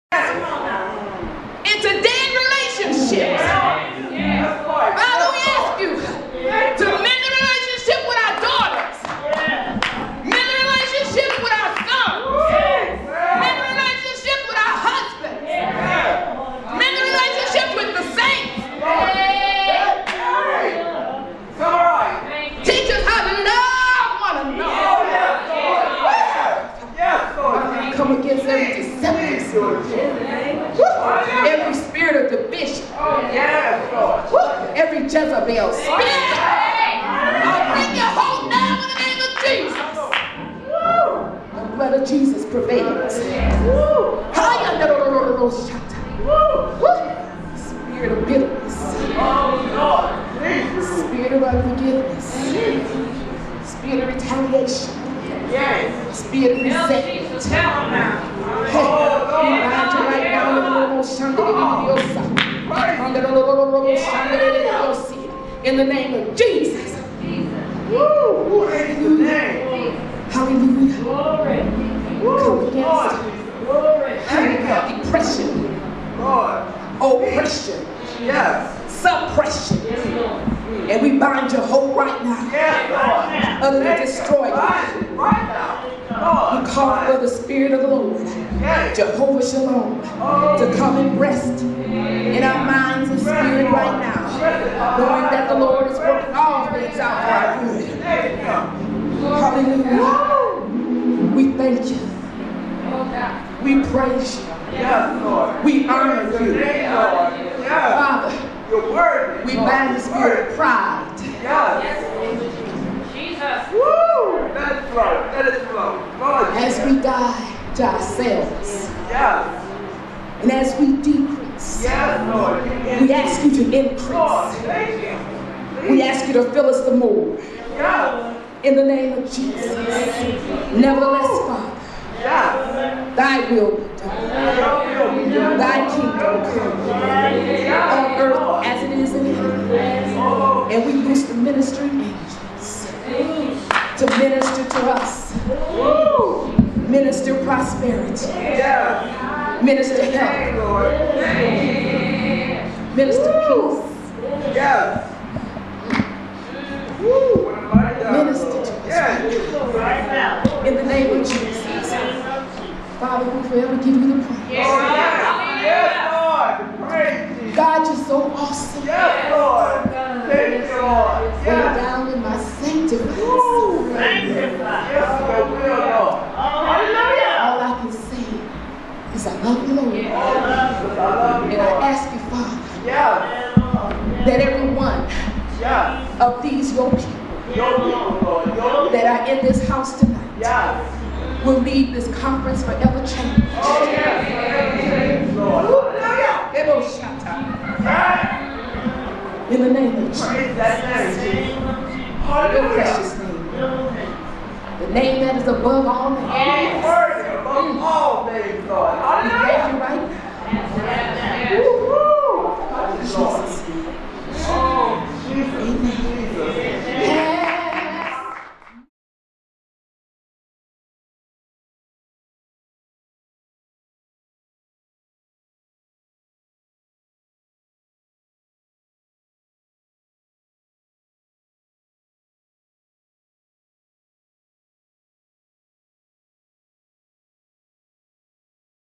Evangelist Ethel Smith "Prayer and Praise"
14th Annual Virtuous Women's Fellowship Conference
July 17, 2008. This Conference was produce by
Pastor Daffany Smith
Life-Line Missions Unlimited, Inc.